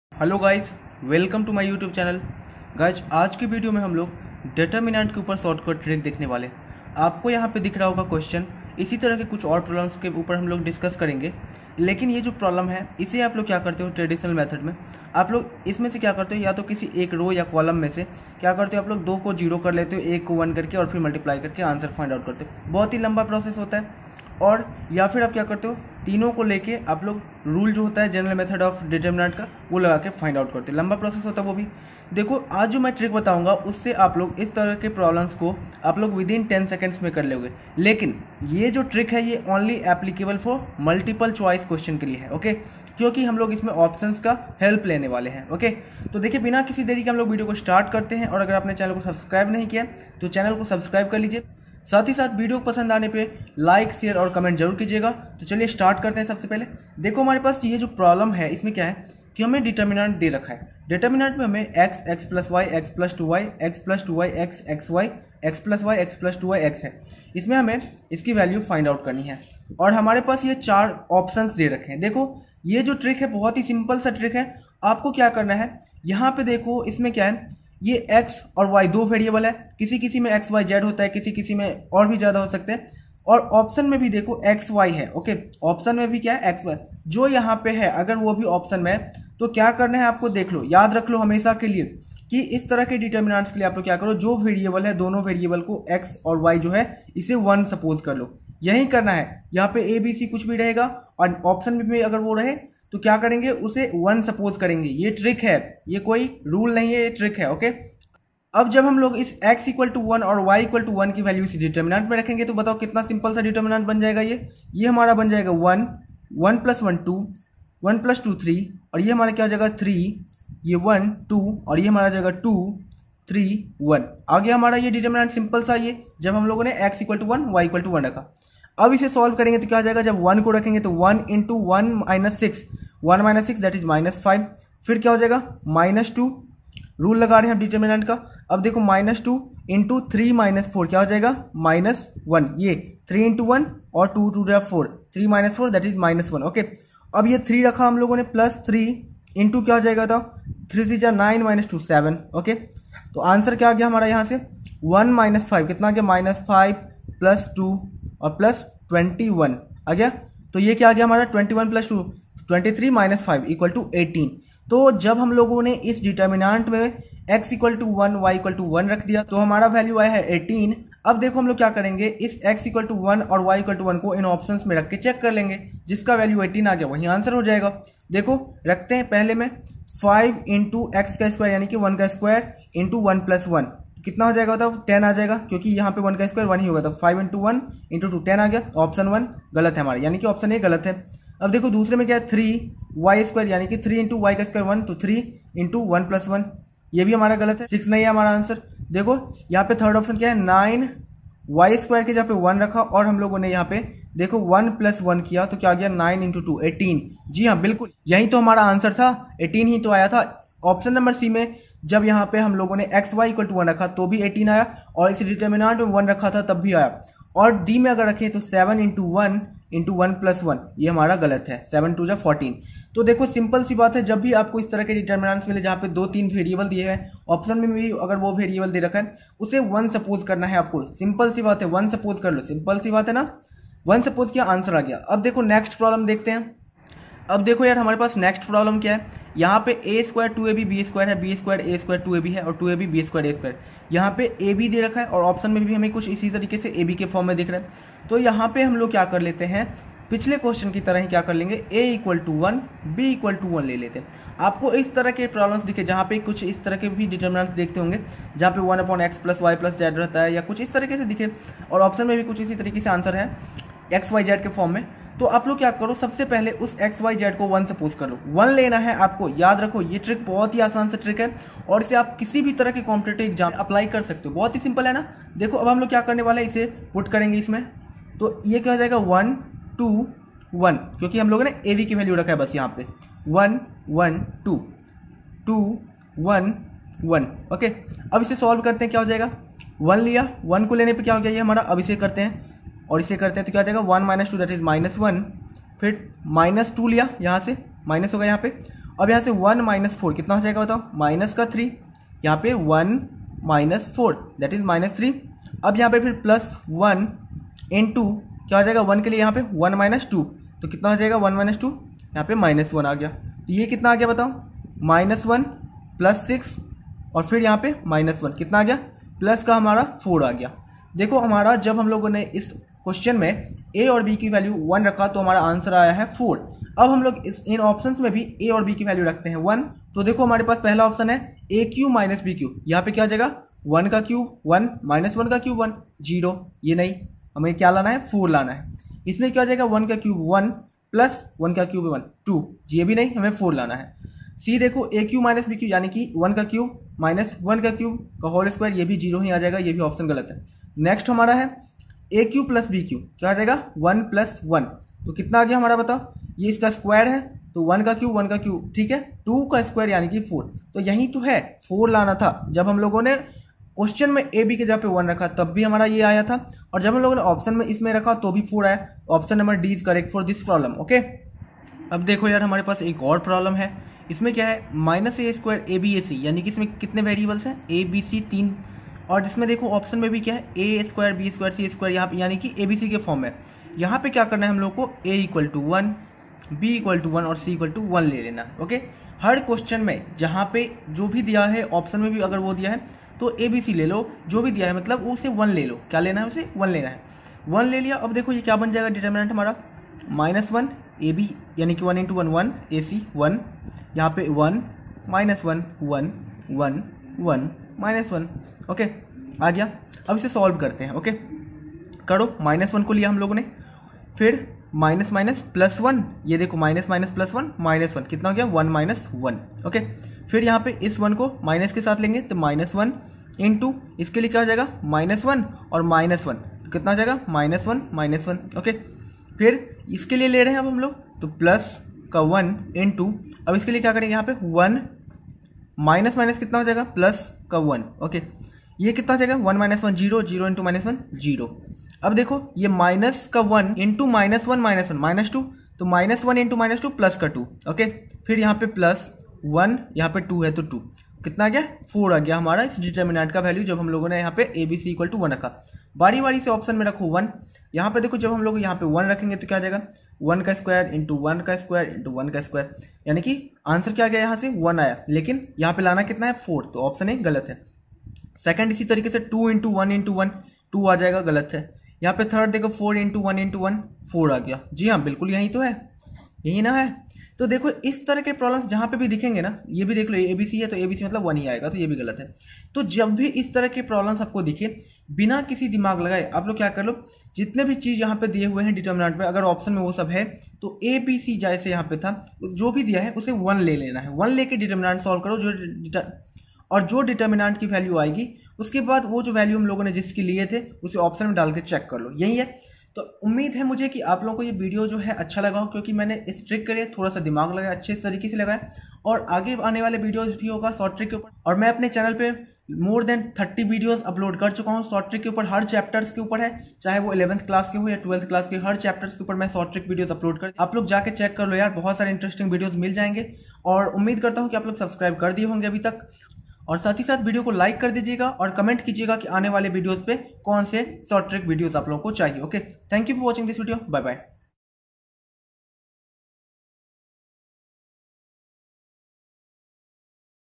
Determinants class 12 shortcut trick || jee/nda/Bitsat/airforce
Here in this video I have revealed a super short trick to solve the problems of determinants within few seconds I hope you will be happy after seeing this